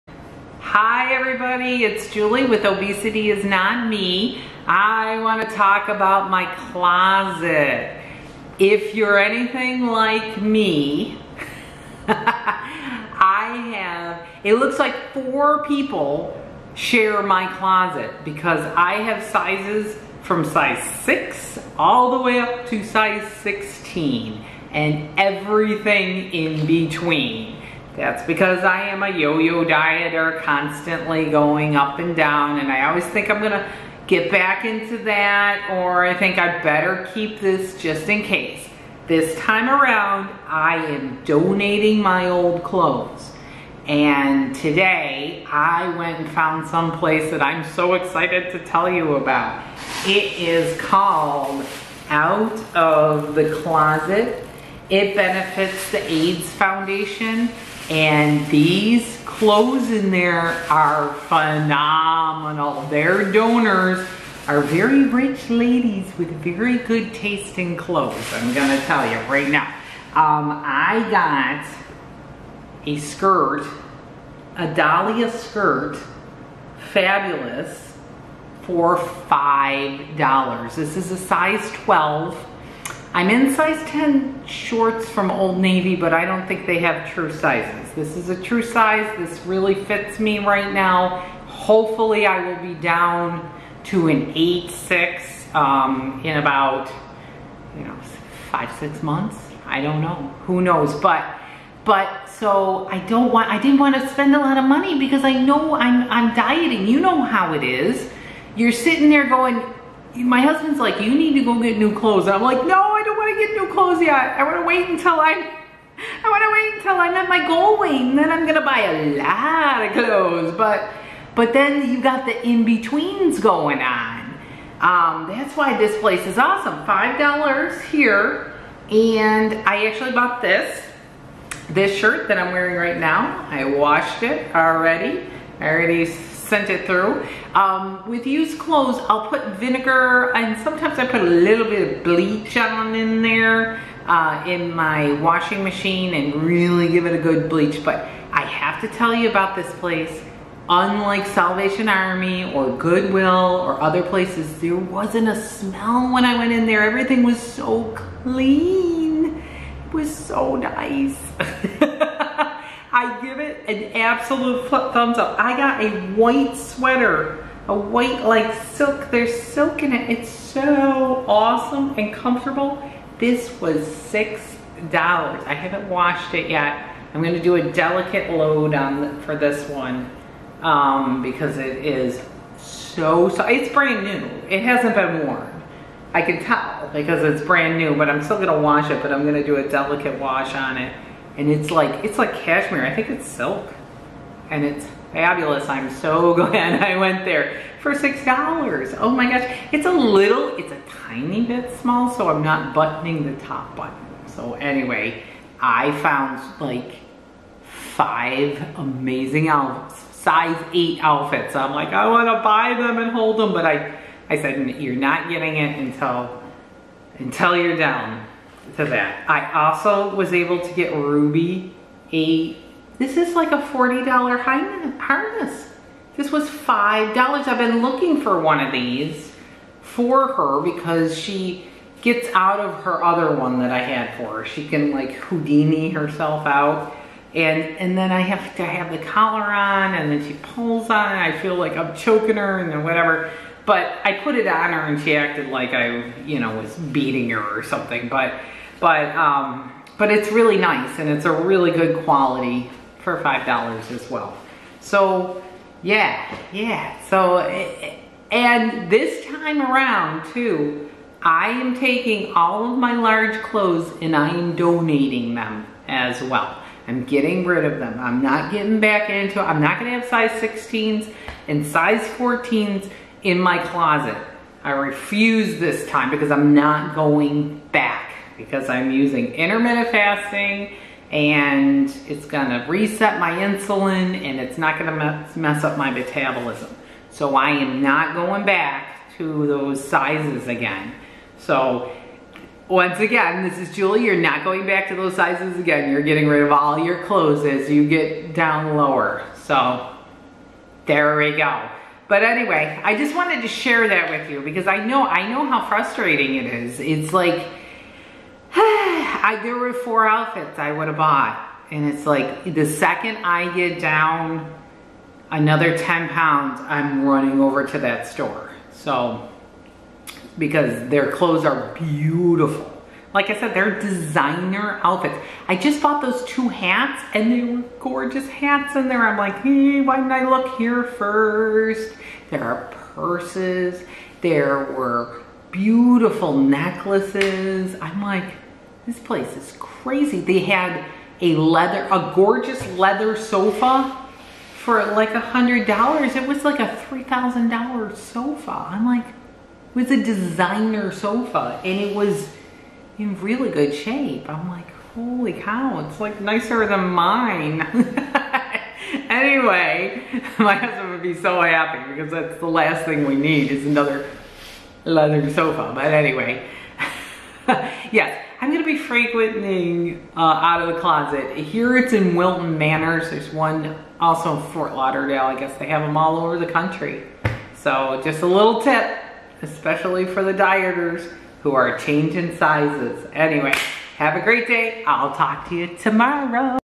Out Of The Closet Thrift Store. Cheap designer in-between the weight loss clothes.
The In-between wardrobe. My closet needs updating without spending a fortune.